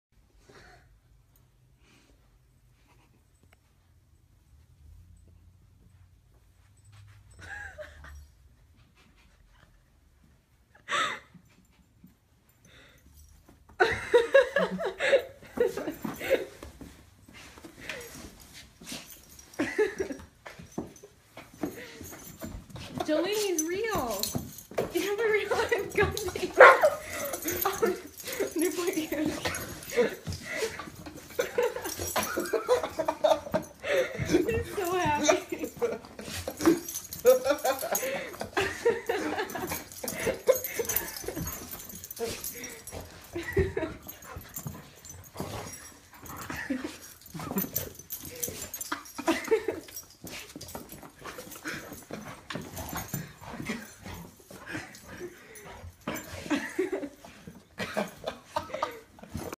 Dono se fantasia de brinquedo preferido de cão e bicho tem ataque de felicidade
Todo cachorrinho tem seu brinquedo favorito. E normalmente é o mais vagabundinho que você compra, depois de gastar fortunas tentando agradá-lo. Jolene, por exemplo, uma fofíssima golden retriever que tem até página no Facebook, escolheu Gumby, a versão de pelúcia de um personagem de animação, como seu melhor amigo. Até que o dono de Jolene fantasiou-se de Gumby e postou-se em frente a Jolene para ver sua reação.

O que ela faria ao encontrar a versão gigante de seu brinquedo? O que aconteceu em seguida você não vai acreditar… Mentira, vai sim, e já está no título. Divirta-se!